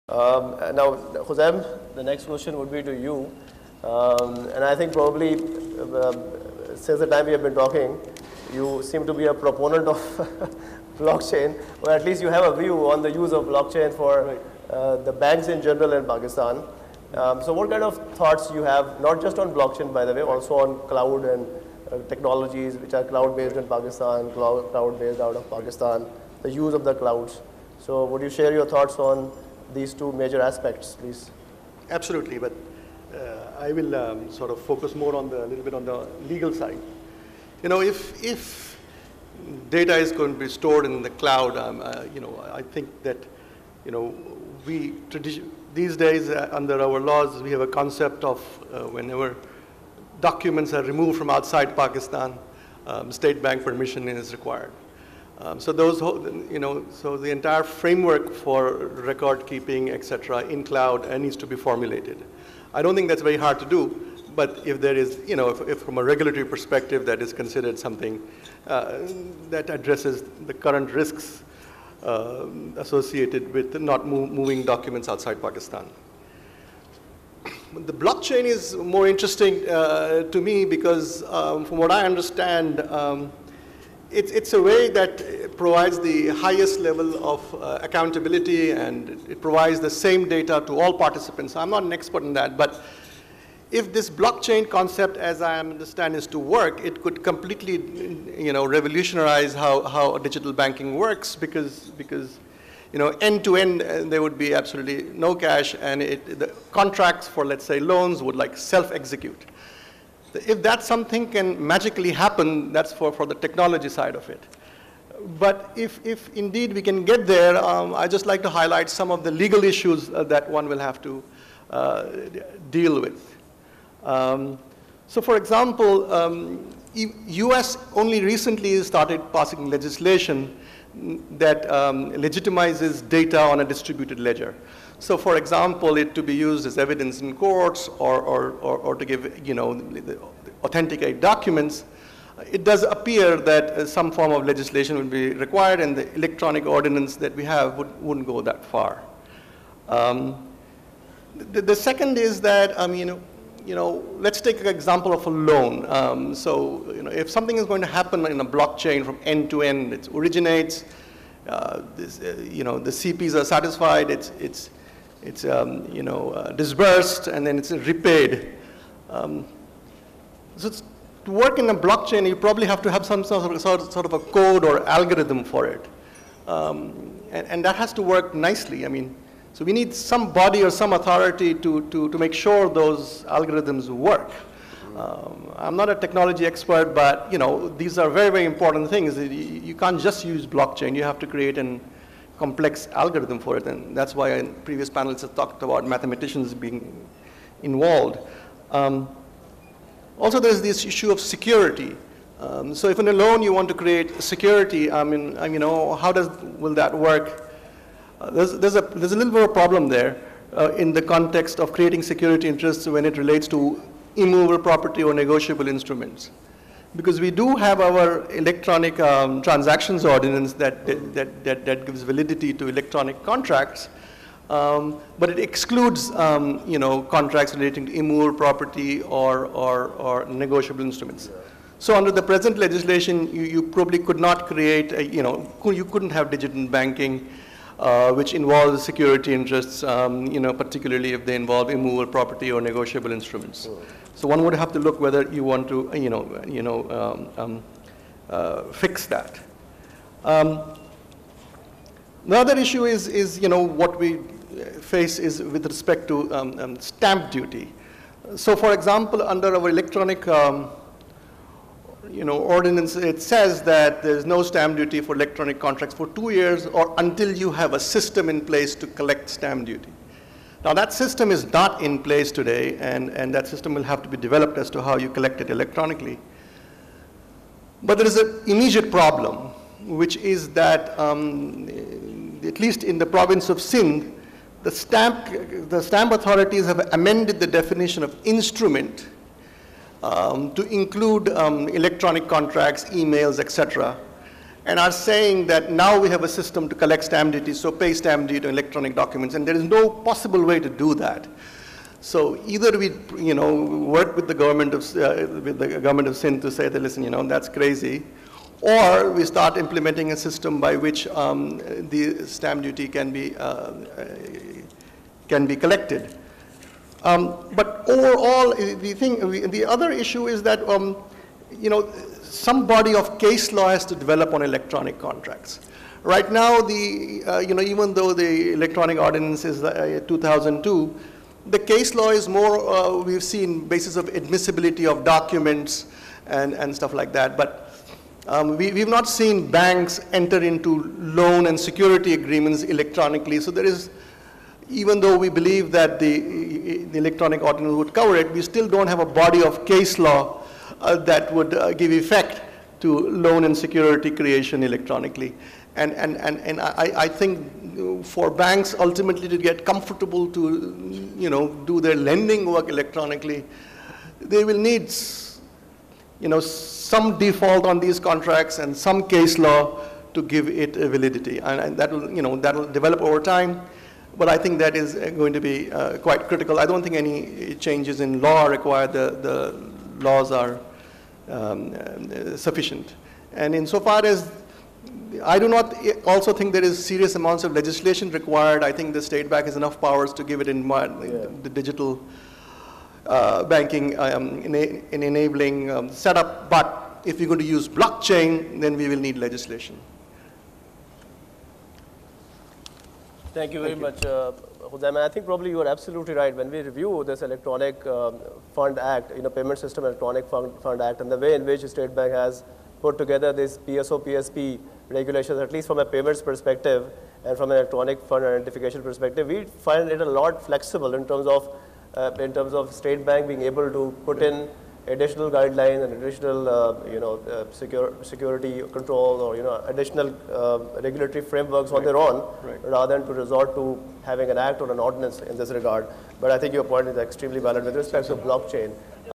017 - Response by Mr. Khozem A. Haidermota (Senior Partner, HaidermottaBNR) in the panel discussion
Response by Mr. Khozem A. Haidermota (Senior Partner, HaidermotaBNR) in the panel discussion on "Regulatory – Legal Sandbox" at the conference on "Digital Banking: The future of Financial Services" held on 24th & 25th April 2018 at Movenpick Hotel Karachi